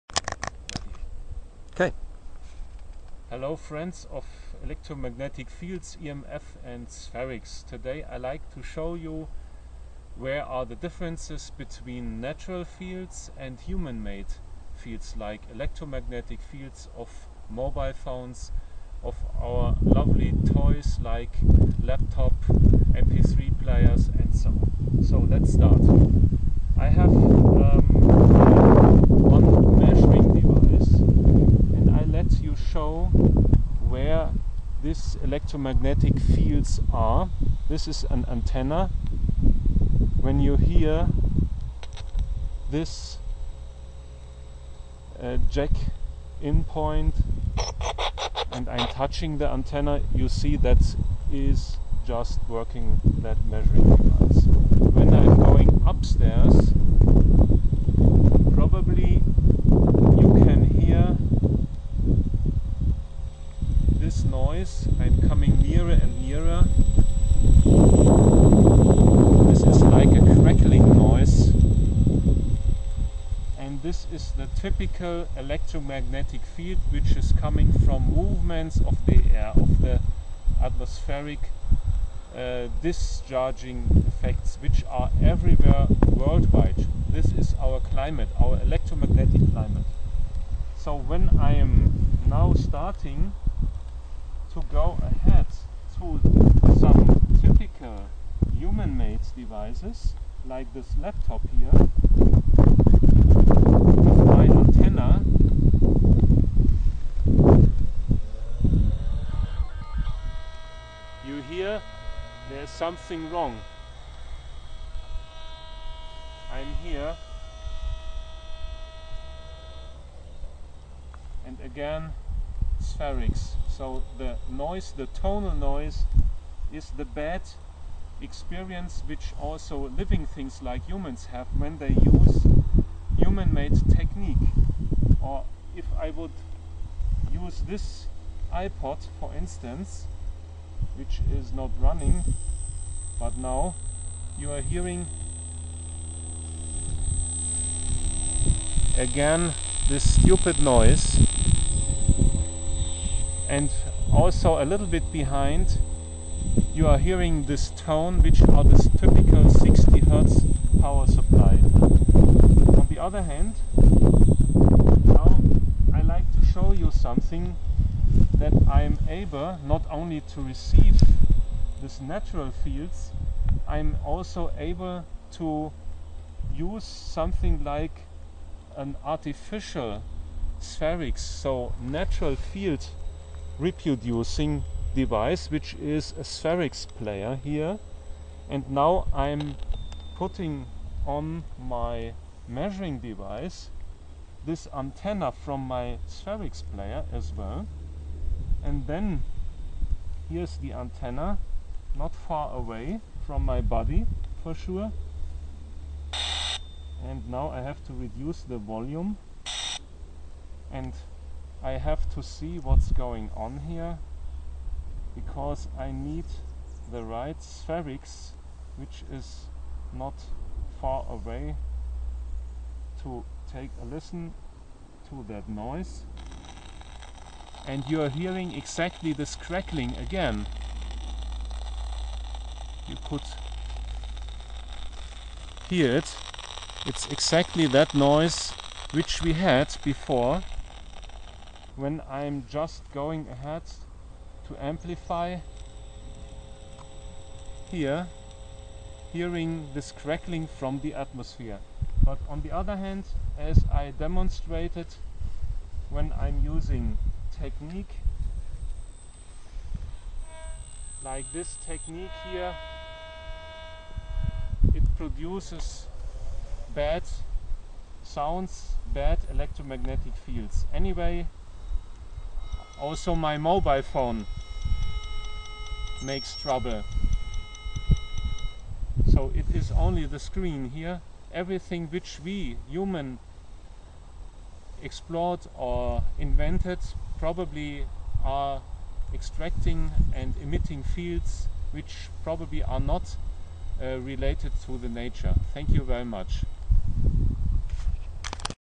sferics-technics EMF comparison measure 27.8.09 elektrosmog
Electro-mag / Elektrosmog, Sferics or natural fields, EMF, distructive technical fields at tech. devices measured and compared live shown in a natural environment - result: It is to use a natural in-coherent field emitter in future wire-less techniques or therapy units - anyway shown the first portable and patented "sferics field emitter" for human well-beings.